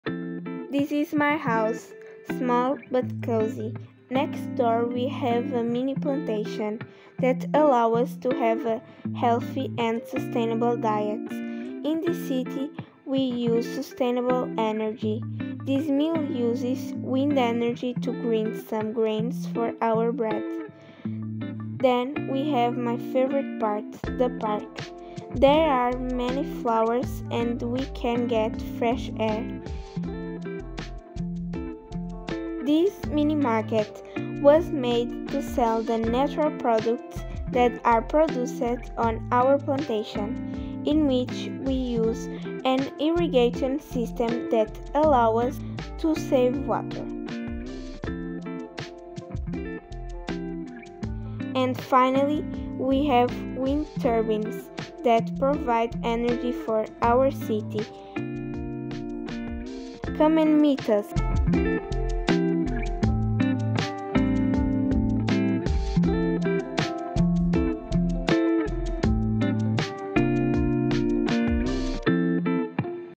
Smart House Carolina & Beatriz
Uma pequena cidade, preocupada com a sustentabilidade ambiental, foi construída por duas alunas do 6ºC, Carolina e Beatriz D. para o projeto eTwinning "Sharing the road" em que participam neste ano letivo.
A small city, concerned with environmental sustainability, was built by two 6ºC students, Carolina and Beatriz D. for the eTwinning project "Sharing the road" in which they are participating this school year.